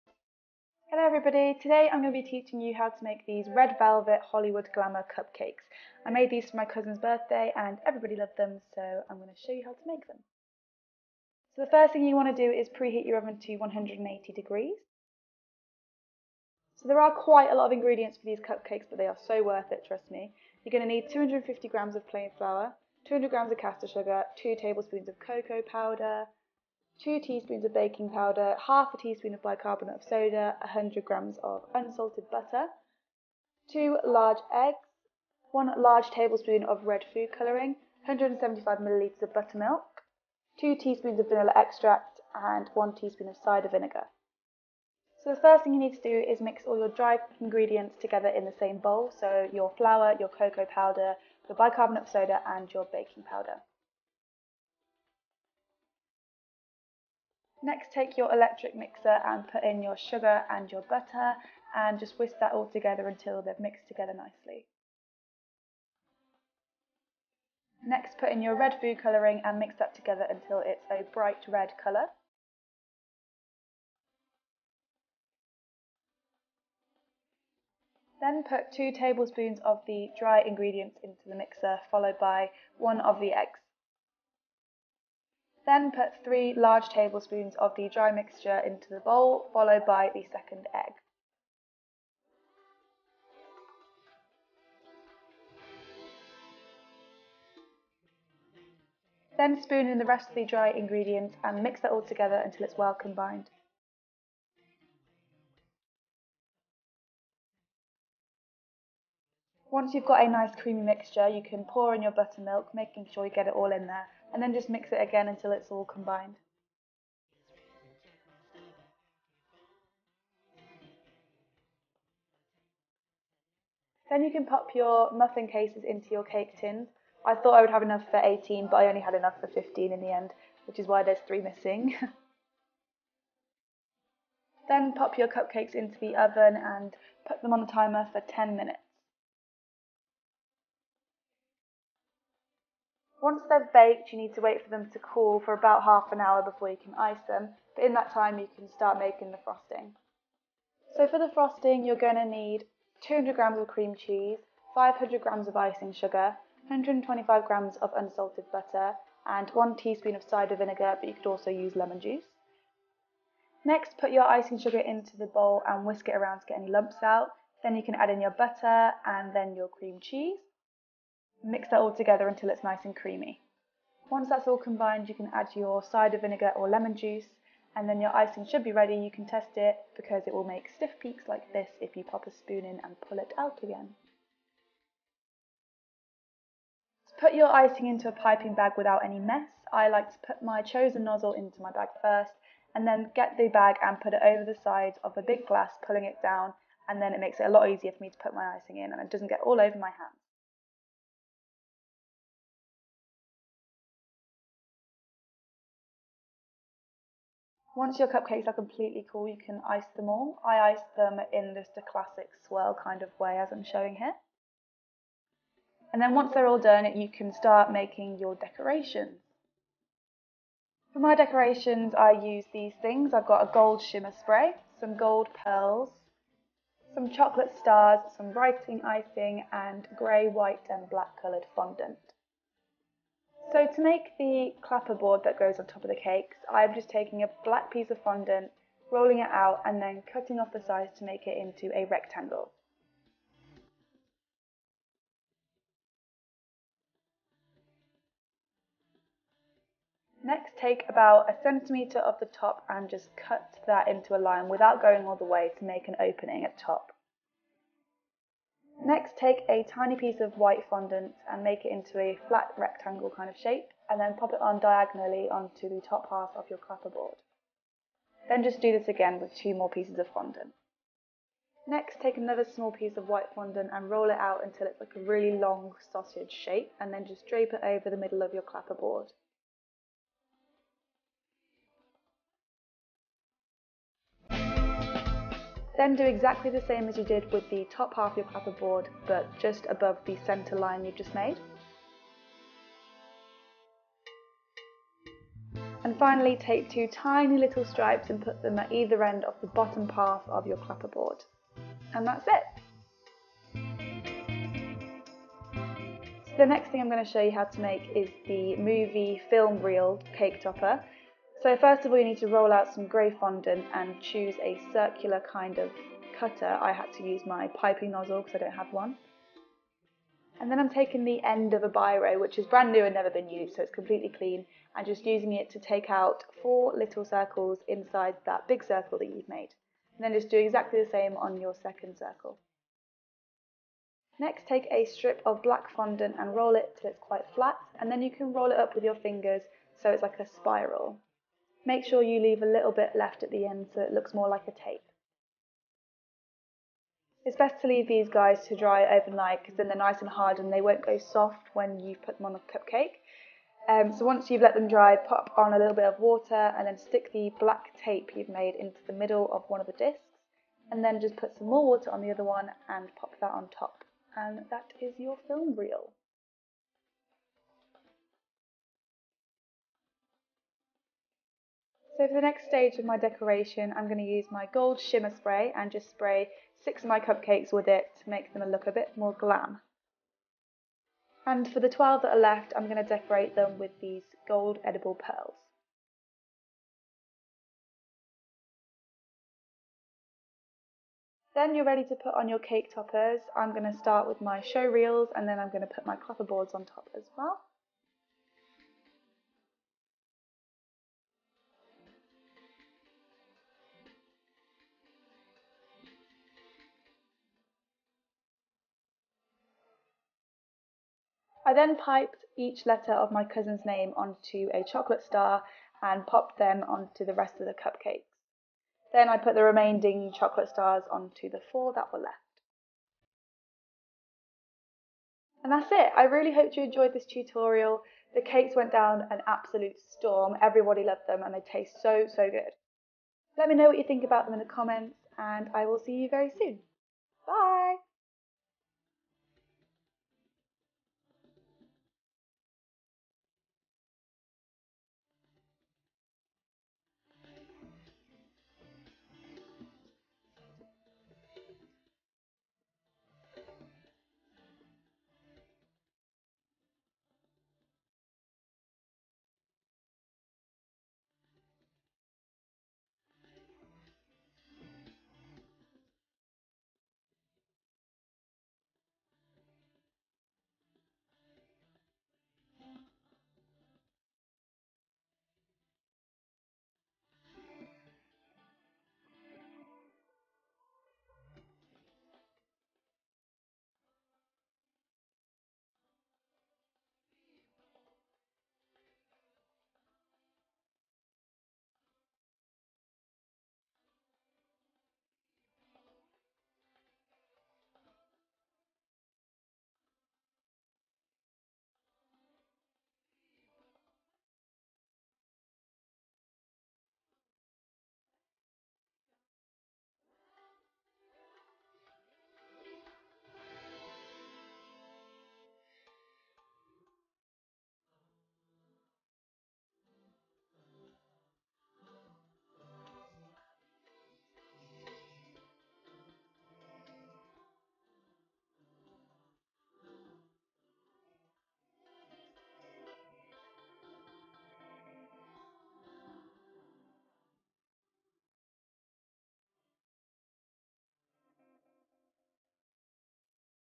Red Velvet Hollywood Theme Cupcakes | ThoseRosieDays
FLIPPING YOUTUBE DIDN'T LIKE THE MUSIC SO THATS WHY IT SOUNDS SO WEIRD.

These Red Velvet cupcakes are my favourite cupcake recipe.
They are so soft and moist and the cream cheese frosting goes perfectly with it.
You can decorate them however you like of course, but as my cousin was throwing a Hollywood Glamour themed birthday party, mine match that theme!
Everyone at the party loved them and I can guarantee you will too.

Measurements:
(US cup conversions in video)

for the cakes:

250 grams plain flour
2 tablespoons cocoa powder (sifted)
2 teaspoons baking powder
½ teaspoon bicarbonate of soda
100 grams soft unsalted butter
200 grams caster sugar
1 heaped tablespoon christmas-red paste food colouring
2 teaspoons vanilla extract
2 large eggs
175 ml buttermilk
1 teaspoon cider vinegar

for the icing:

500 grams icing sugar
125 grams cream cheese
125 grams soft unsalted butter
1 teaspoon cider vinegar (or lemon juice)